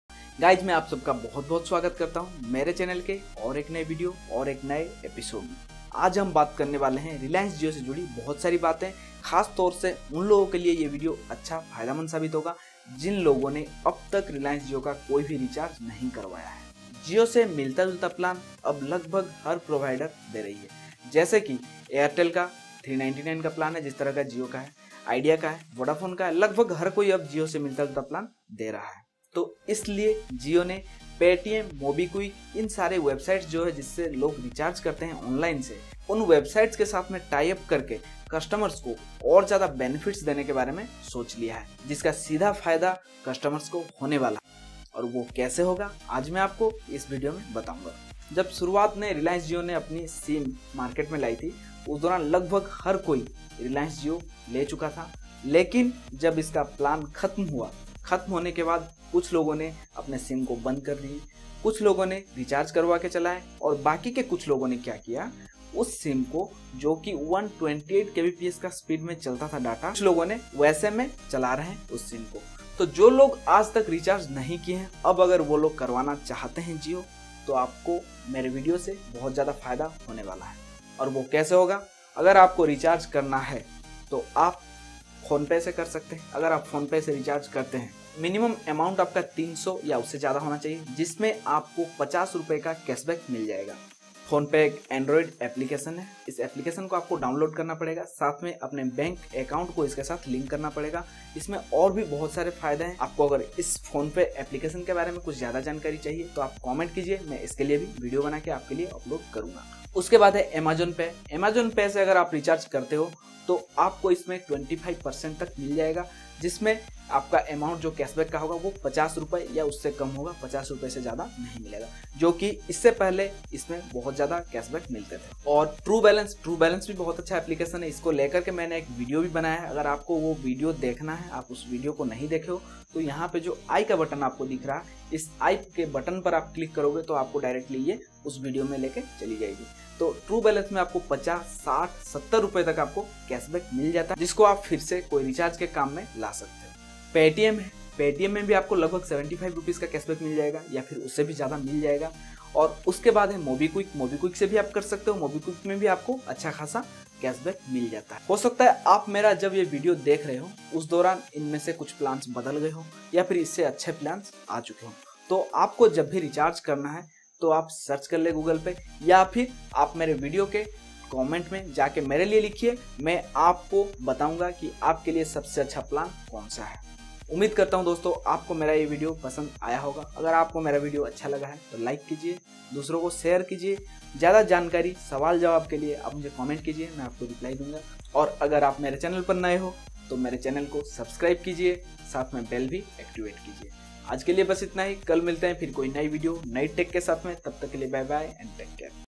जिओ का रिचार्ज अबतक नहीं किया ? डरो मत खुसखबरी है आपकेलिये !!
जो लोग जिओ के अबतक कोई भी रिचार्ज नहीं किये है वो इस विडिओ को जरुर देखे, क्यों की अगर अब तक आपने रिचार्ज नहीं किया तो अब आपको सस्ते दामो में वही प्लान मिलने वाले है , बीडीओ को पूरा देखिये और मुझे कमेन्ट करके बताइए आपको कैसा लगा बीडीओ , अच्छा लगे तो लाईक शेयर और हमारे चेनेल को सबस्क्राइब करना न भूले .